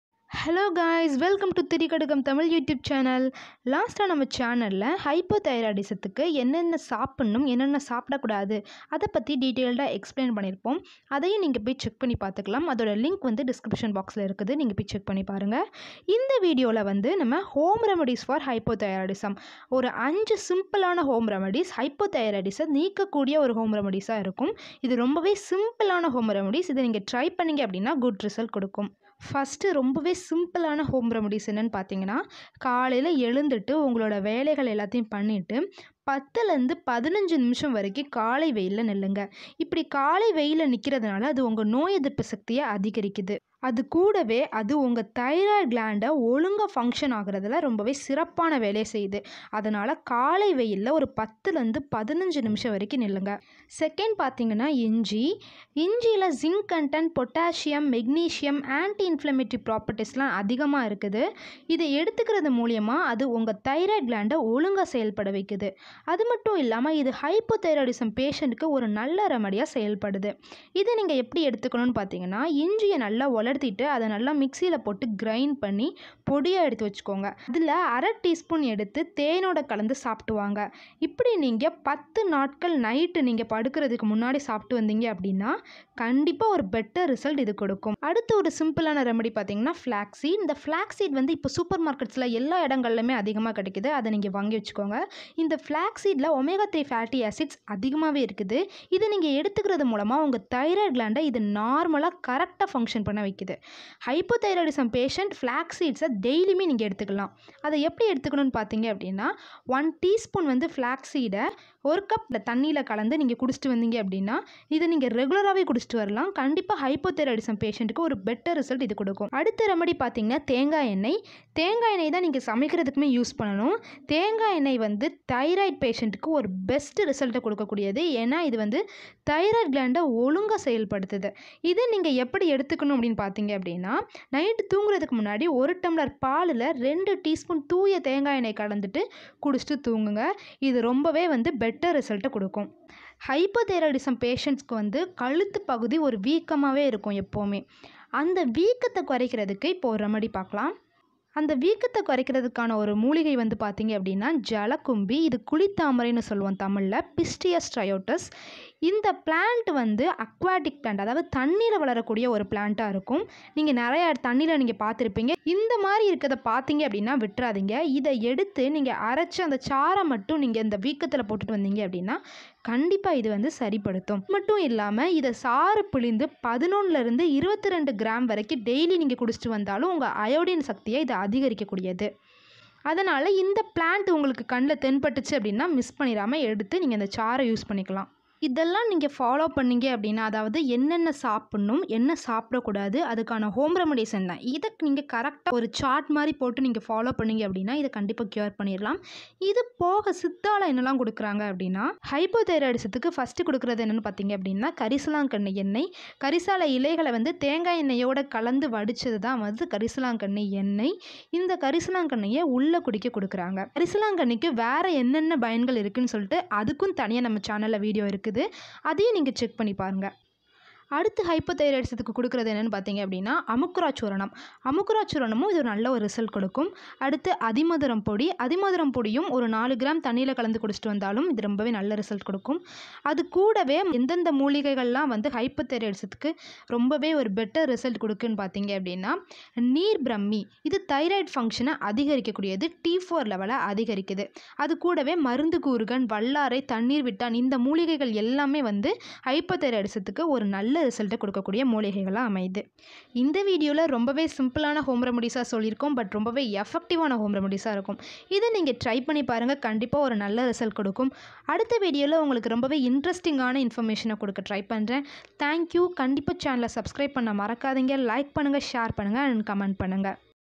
Simple home remedies and siddha medicines for hypothyroidism/Thirigadugam tamil
This video tells about simple remedies and siddha medicines for hypothyroidism.
This channel gives you the benifits of siddha medicines.